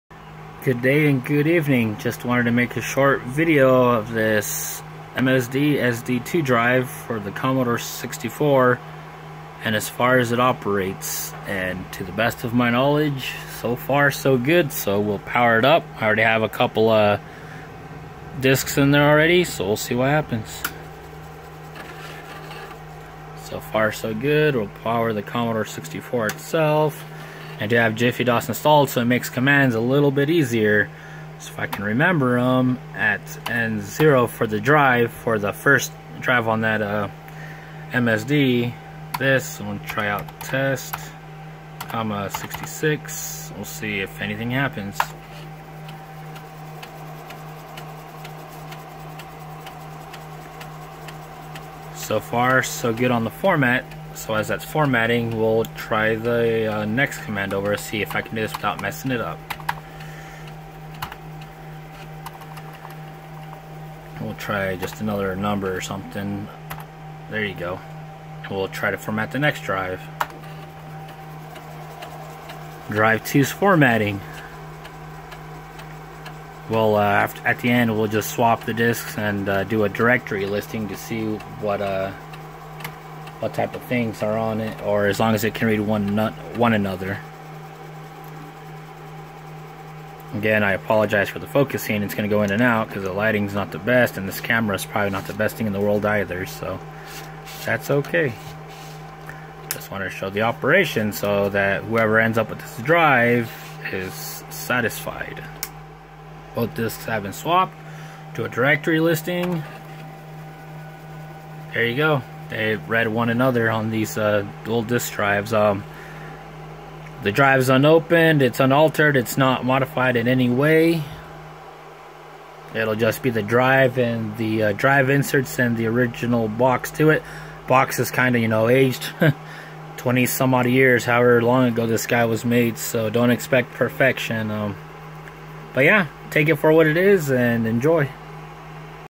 MSD SD-2 Dual Disk Drive for the Commodore 64 and other 8-bit Computers
Just tested out a MSD SD-2 disk drive I acquired.  Wanted to show proof of it working before it gets listed on eBay.